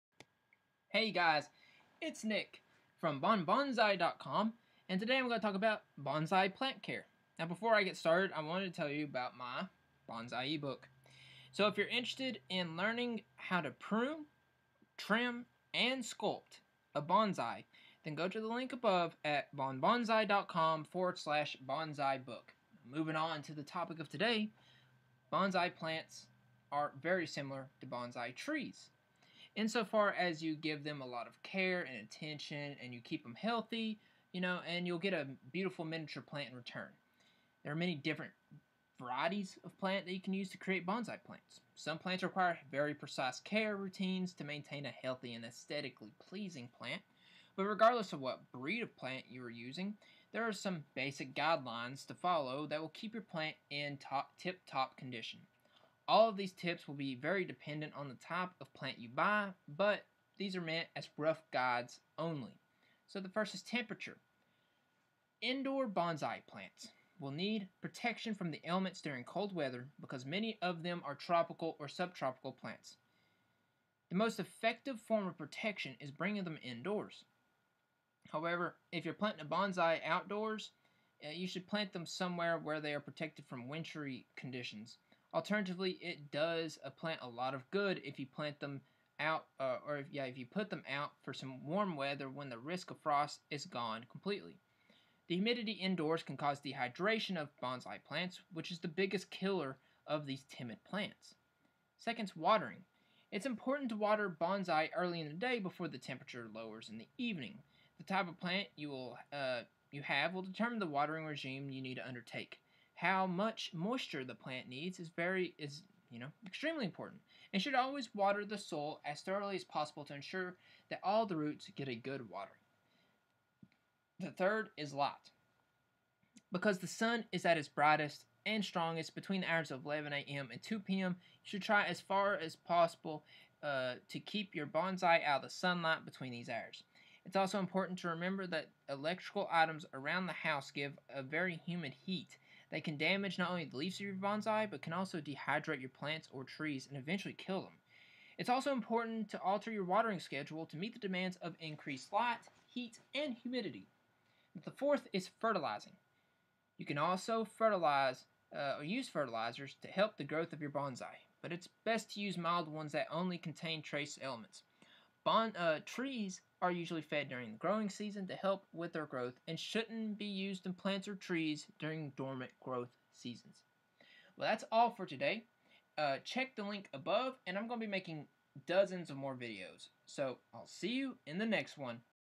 Bonsai Plant Care
About this video:
Bonsai plants are very similar to Bonsai trees in so far as if you give them a lot of care and attention and keep them healthy you will get a beautiful miniature plant in return. There are many different varieties of plant that you can use to create Bonsai plants. Some plants require very precise care routines to maintain a healthy and aesthetically pleasing plant, but regardless of what breed of plant you are using there are some basic guidelines to follow that will keep your plant in condition. All of these tips will be dependant on the type of plant you buy but are meant as rough guides only.

Temperature.

Indoor Bonsai plants will need protection from the elements during cold weather because many of them are tropical or subtropical plants. The singly most effective form of protection is bringing them indoors. However, if you are planting your Bonsai outdoors you should plant them somewhere where they are protected from wintry conditions. Alternatively it does a plant a lot of good if you put them out for some warm weather when the risk of frost is gone completely. The humidity indoors can cause dehydration of Bonsai plants, which is the biggest killer of these timid plants.

Watering.

It is important to water Bonsai early in the day before the temperature lowers in the evening. The type of plant you have will determine the watering regime you need to undertake. How much moisture the plant needs is a very important factor and you should always water the soil as thoroughly as possible to ensure that all the roots get a good watering.

Light.

Because the sun is at its brightest and strongest between the hours of 11 a.m. and 2 p.m. you should try as far as possible to keep your Bonsai out of the sunlight between these hours. It is also important to remember that electrical items around the house give off a very humid heat that can damage not only the leaves of you Bonsai but can also dehydrate your plants or trees and eventually kill them. Its also important to alter your watering schedule to meet the demands of increased light, heat and humidity.

Fertilizing.

You can use fertilizers to help the growth of your Bonsai plants, but its best to use mild ones that only contain trace elements. Trees are usually fed during the growing season to help with their growth and shouldnt be used in either plants or trees during dormant growth periods.